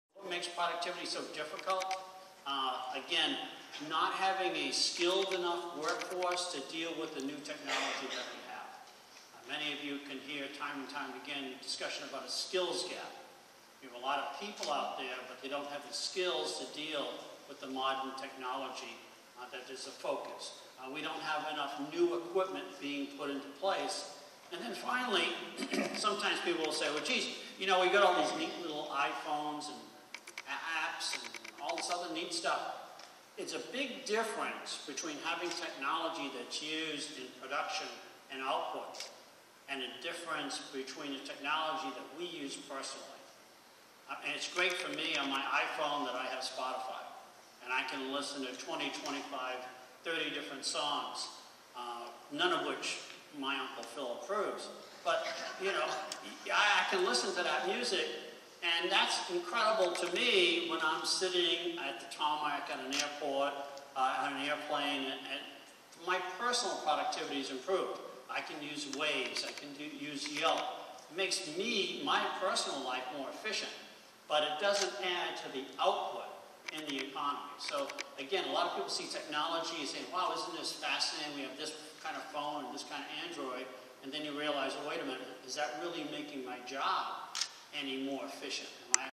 Wells Fargo Chief Economist John Silvia talks about the "skills gap."
By: hhayes1
Published on: October 19, 2017